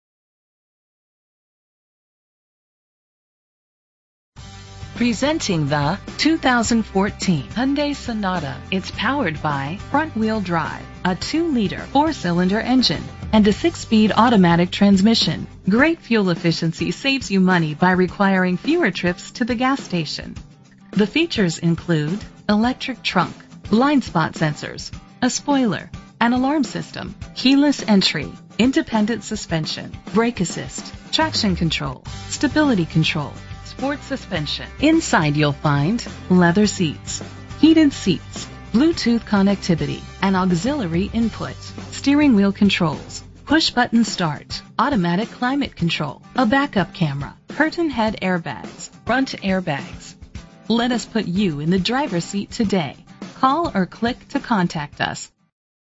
2014 Hyundai Sonata 12076A - Sevierville TN
Year: 2014
Make: Hyundai
Model: Sonata
Trim: SE 2.0T
Engine: 2.0 liter 4 Cylindercylinder FWD
Transmission: 6-Speed Shiftable Automatic
Color: White
Mileage: 47185
Address:
VIN:5NPEC4AB9EH903682